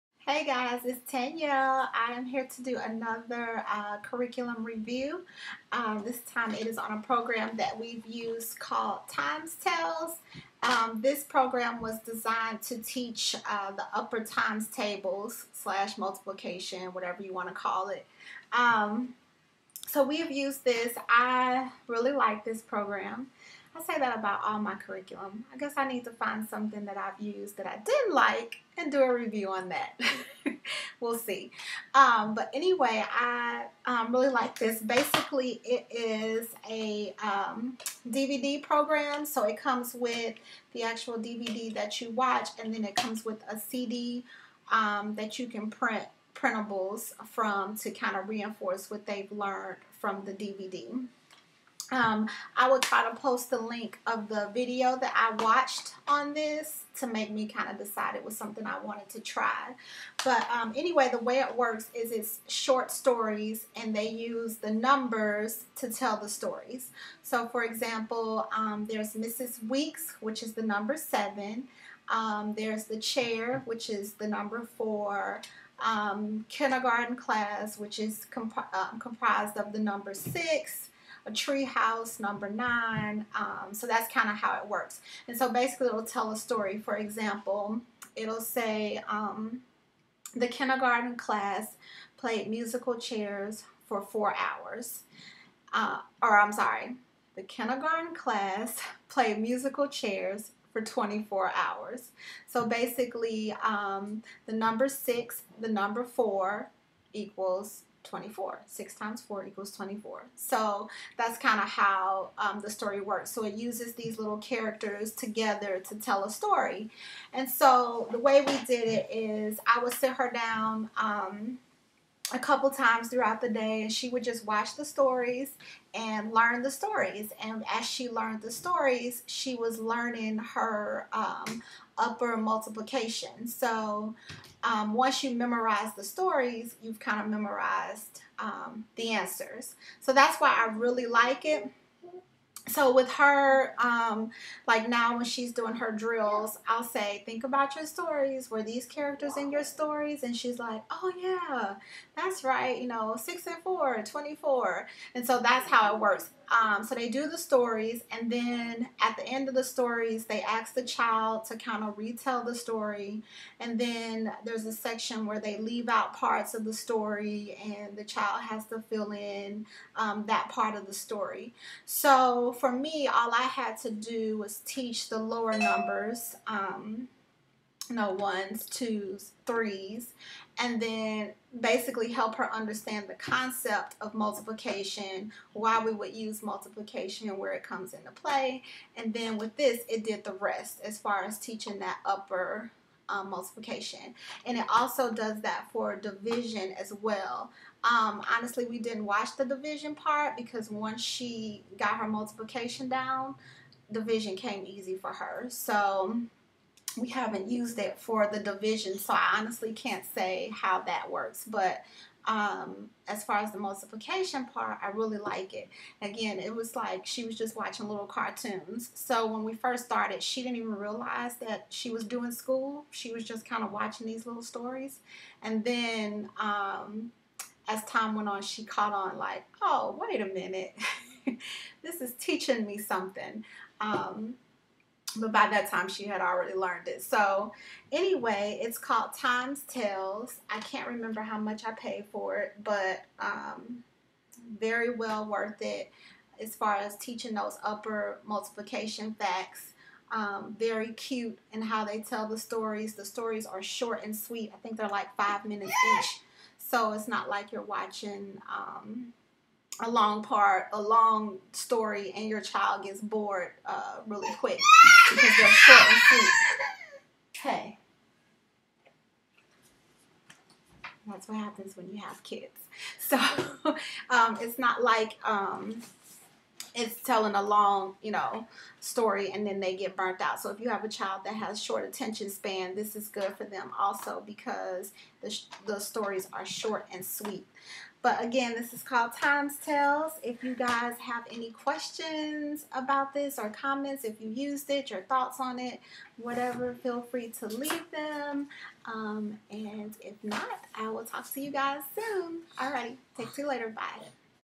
Review On Times Tales Multiplication CD. Homeschool math curriculum.
Short review on the curriculum/program times tales.